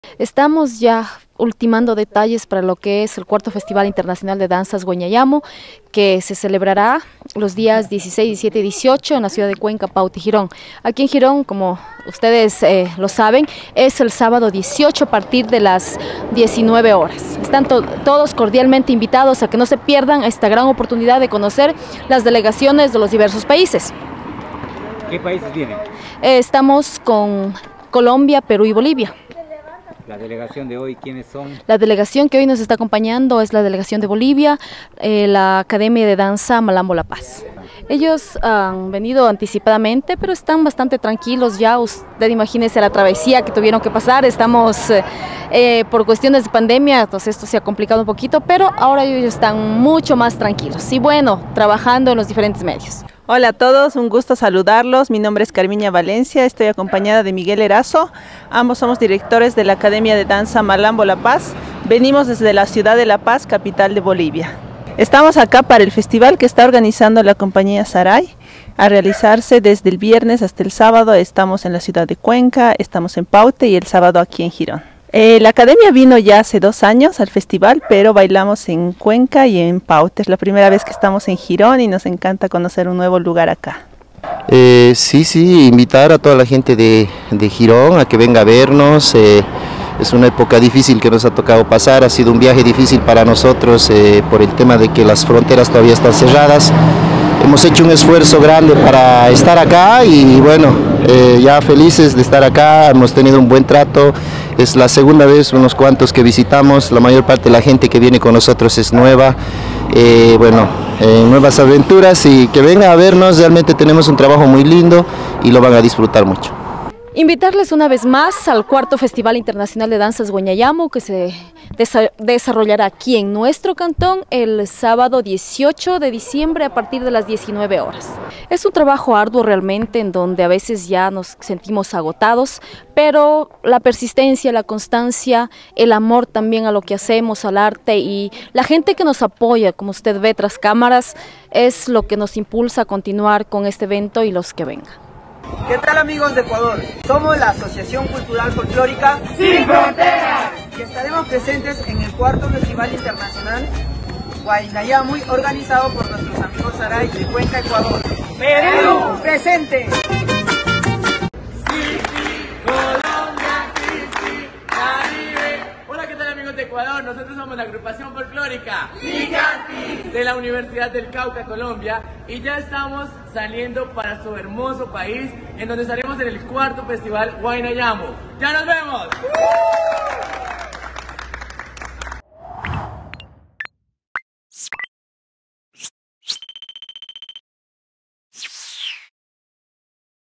DANZAS INVITACION
Este fin de semana Girón será sede del cuarto festival internacional de danza

La Compañía Integral de Danzas Saray de la ciudad de Cuenca, ultima detalles para la presentación del Cuarto Festival Internacional de Danza, donde el cantón Girón ha sido escogido por los promotores para que sea sede. El sábado 18 de diciembre desde las 19h00 en el parque “25 de Junio” se cumplirá el evento cultural con la participación de delegaciones de Colombia, Perú, Bolivia y Ecuador.